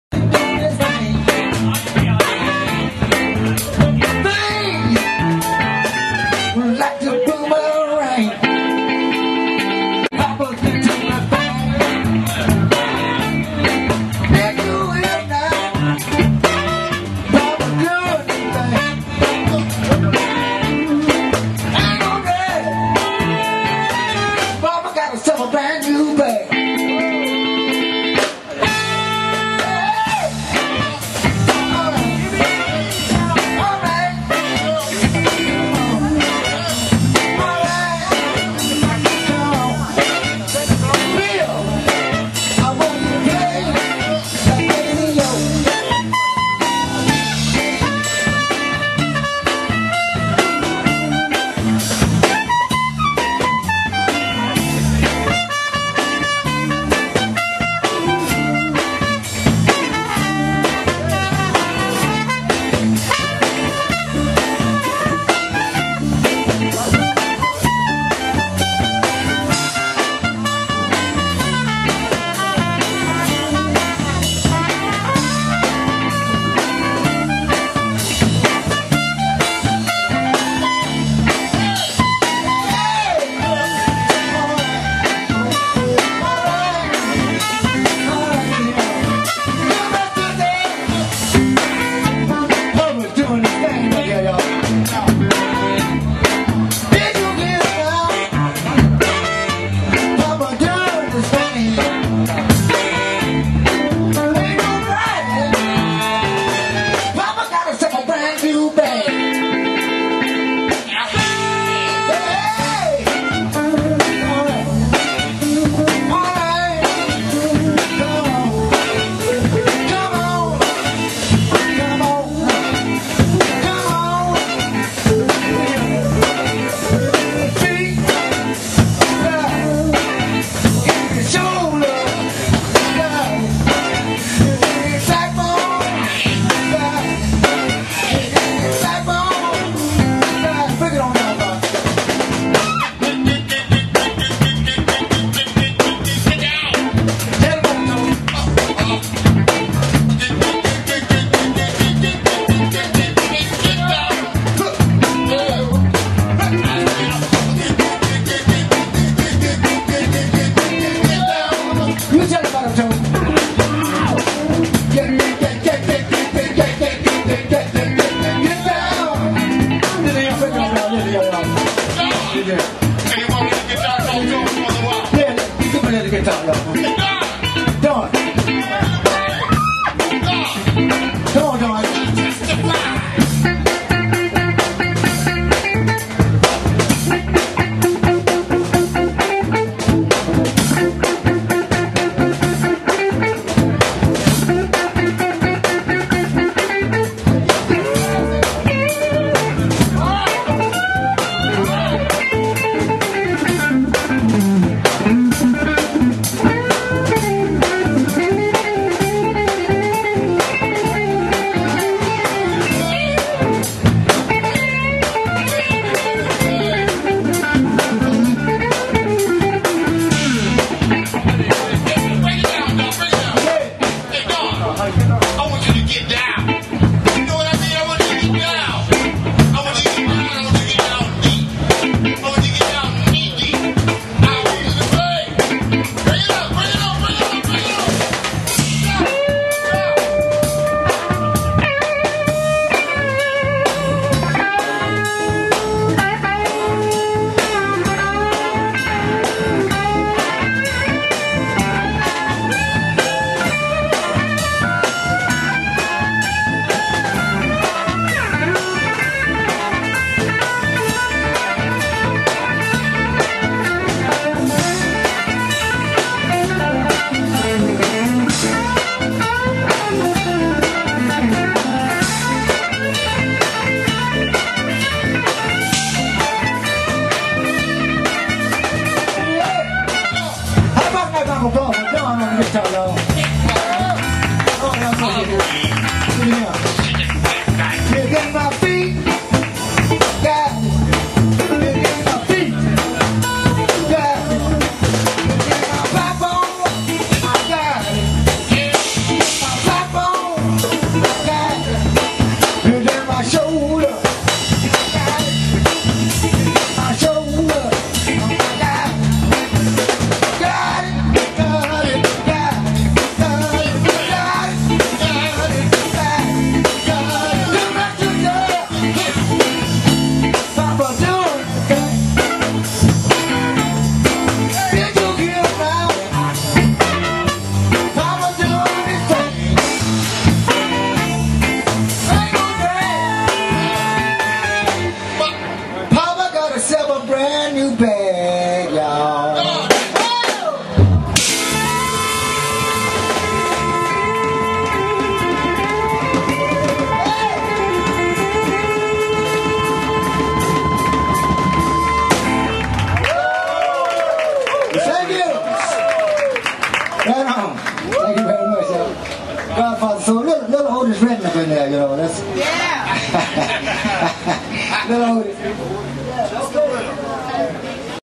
Jimmie Lee Moore and the James Brown Band - Van Dyke's Cafe Pt. 1
My friend Jimmie Lee Moore was the bass player in James Brown's band for twenty years. This is a video of the band playing in Miami Beach, in the current format which is The James Brown Tribute Band (minus the actual Godfather of Soul, obviously). They still rock after all these years and Jimmie Lee plays with me in the Ike and Val Woods RnB Revue.